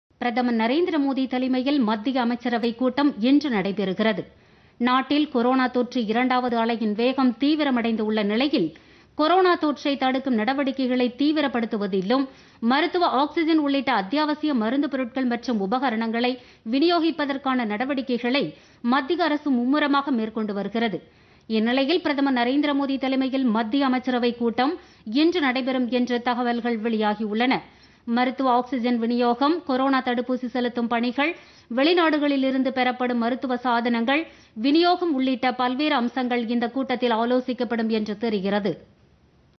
மத்திய அமைச்சரவை ஆலோசனை
பிரதமர் நரேந்திர மோதி தலைமையில் மத்திய அமைச்சரவை இன்று ஆலோசனை - கொரோனா தடுப்பு நடவடிக்கை, ஆக்ஸிஜன் விநியோகம் குறித்து ஆய்வு.